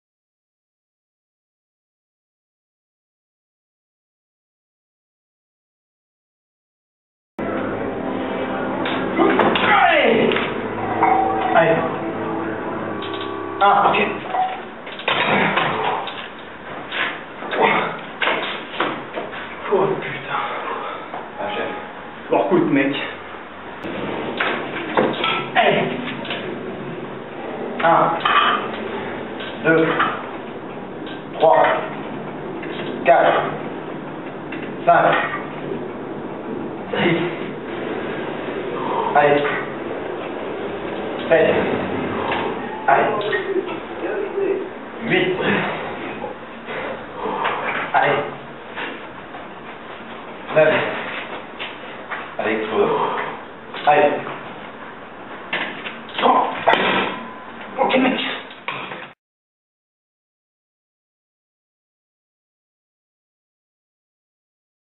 Yann S. - 1@150 kg - 9@130 kg au développé couché
La Team SuperPhysique vous présente Yann S. au développé couché.